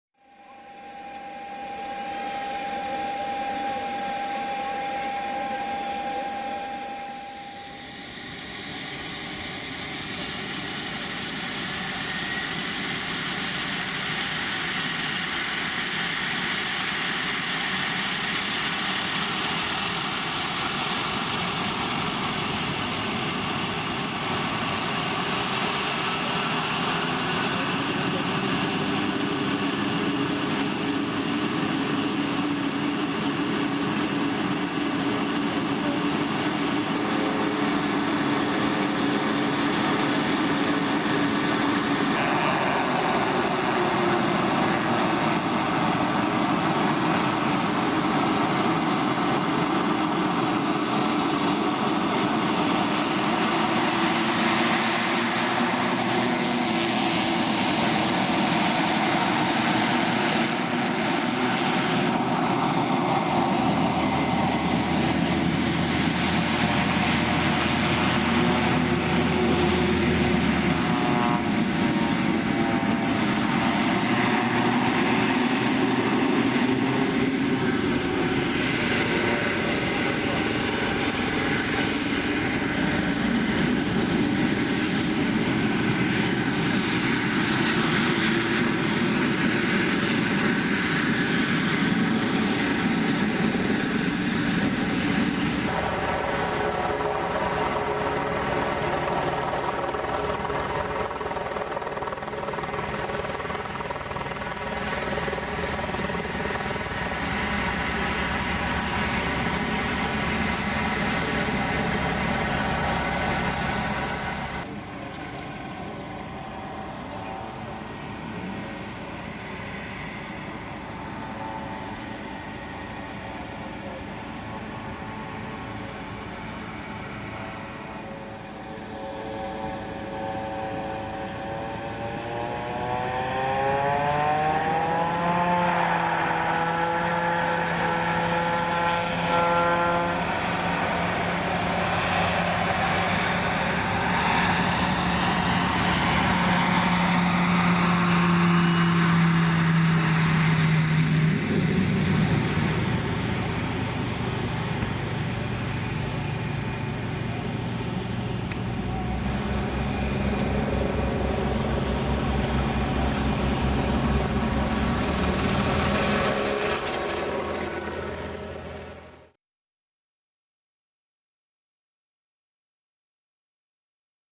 Piper PA-46-500TP takeoff at St. Moritz / Samedan Airport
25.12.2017 - Piper PA46 Malibu Meridian | N203AP
Taxiing on the snow paved apron, then takeoff on runway 03

Rollen auf dem Natur-Vorfeld und Start auf der Piste 03